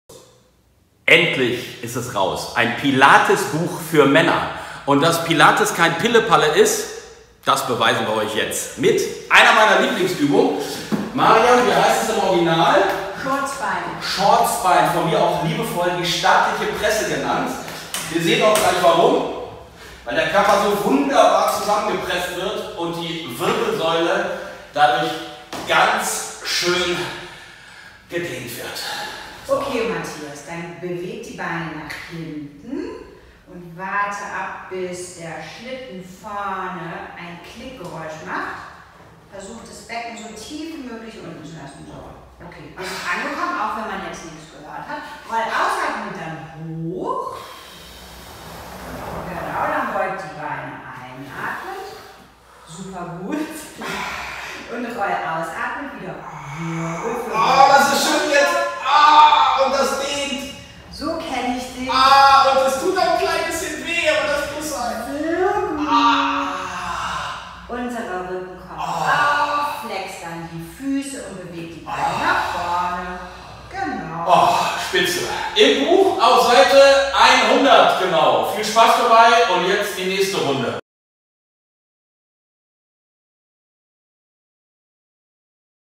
Pilates für Männer mit Matthias Opdenhövel- Übung 1
3.9.2018 im Handel und als ebook

Pilates-Fitness für Männer - bestechend einfach und extrem effektiv.

Der TV-Moderator Matthias Opdenhövel präsentiert zusammen mit seiner Trainerin Mariam Younossi das Pilates-Übungsbuch für Männer.

Für Matthias Opdenhövel ist Pilates eine der effektivsten Sportarten. Es bietet Männern optimale Übungen für eine ganzheitliche Fitness. „Jeder Mann sollte Pilates machen! Denn das ist kein Pillepalle, sondern das beste Ganzkörper-Training, das ich kenne.“

Das mit seiner Trainerin Mariam Younossi ursprünglich auf ihn abgestimmte Programm ist nicht nur die beste Prophylaxe, damit Beschwerden erst gar nicht entstehen, sondern mobilisiert und stabilisiert optimal alle Bereiche des Körpers. Das Training fördert die Gesundheit, Beweglichkeit und Balance, Verletzungen werden vermieden, die Haltung verbessert und man entwickelt eine ganz natürliche Ausstrahlung.

Die 52 Übungen werden an Pilates-Geräten von Matthias Opdenhövel gezeigt und ergänzt mit „Pilates to go“ auf der Matte. Die Wirkung jeder Übung wird erklärt und durch die persönlichen Erfahrungen des Sport-Moderators für jeden Mann zugänglich.